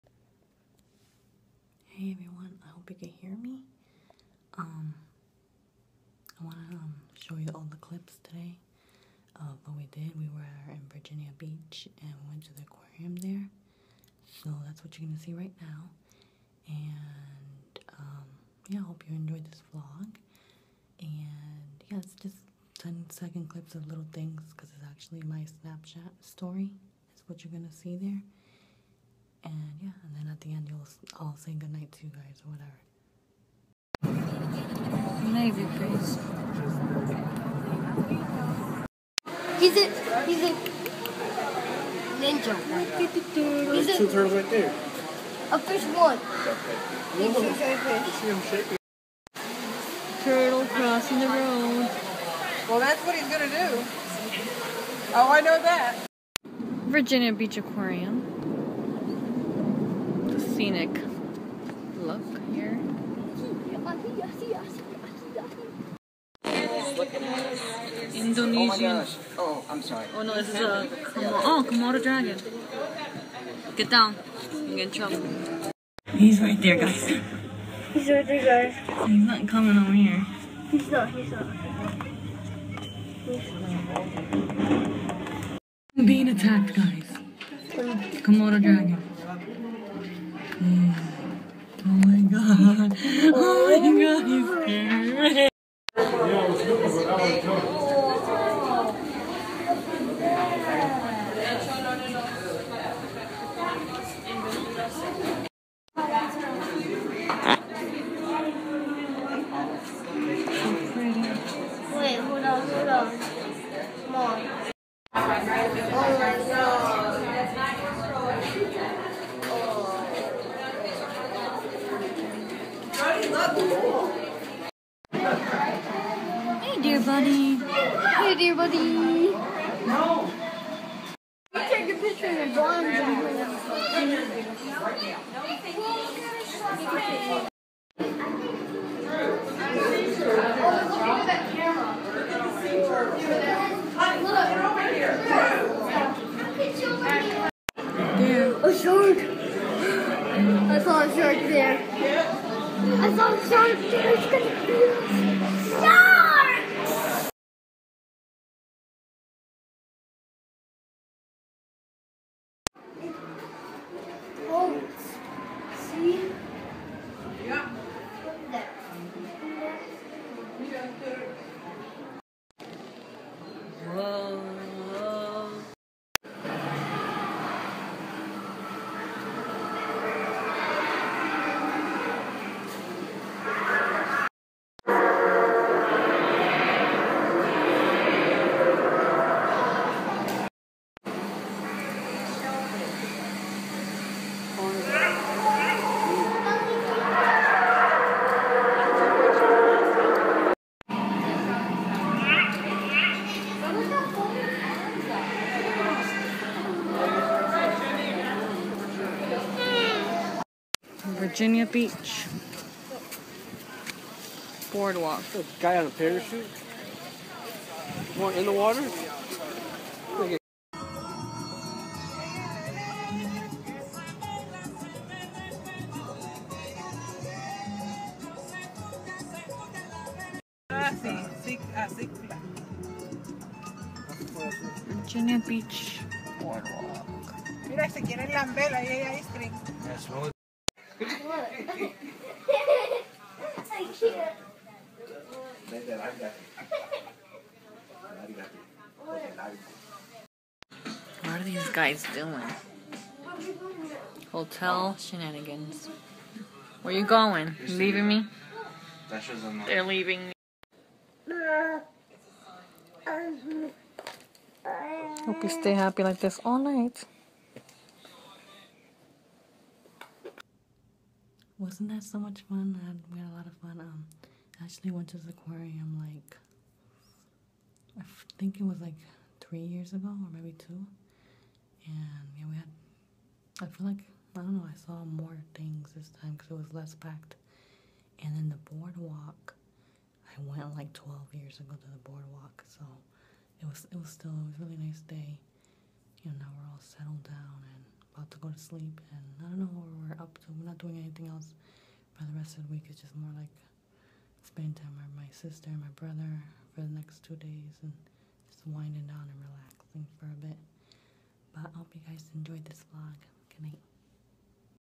Virginia Beach Aquarium & Boardwalk
Quick snapchat story of our day at Virginia Beach aquarium & Boardwalk.